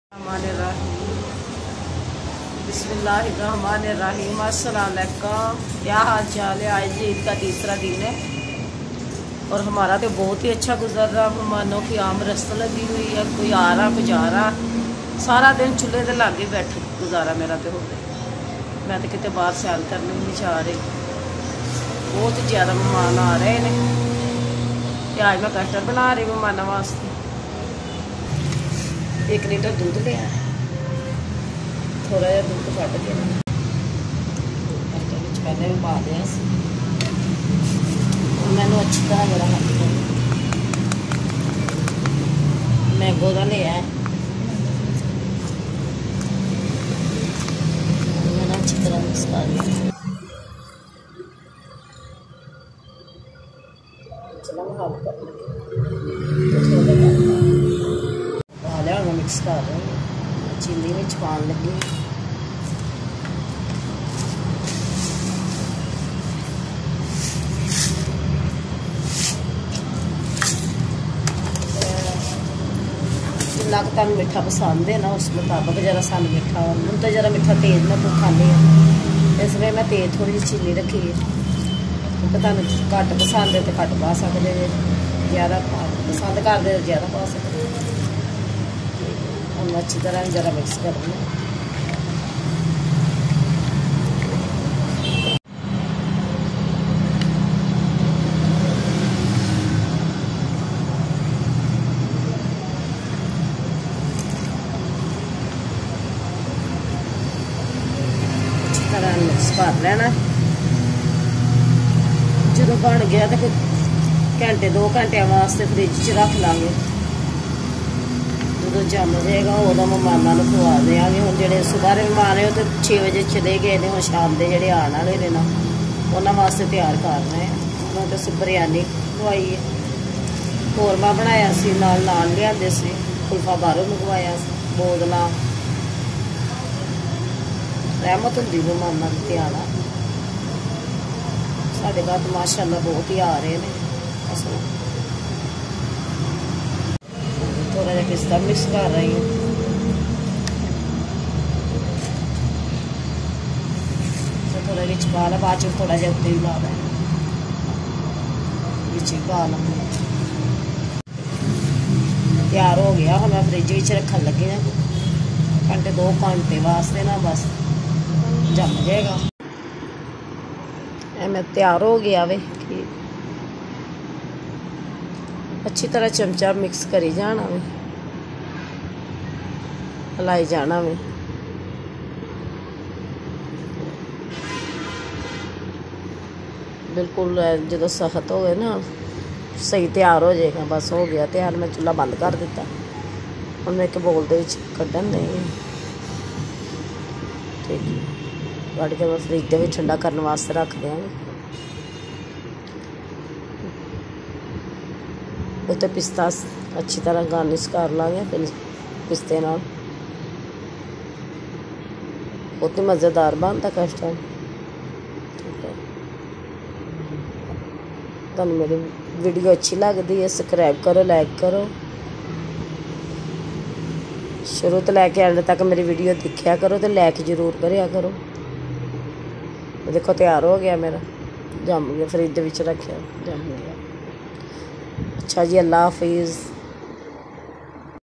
How to make Mango Custard | Tasty Recipe Mango Kheer
Mango custard is very good and you must make it at home.

OTHER RECIPE,
How to make potato peas goll roll, Samosay roll click here.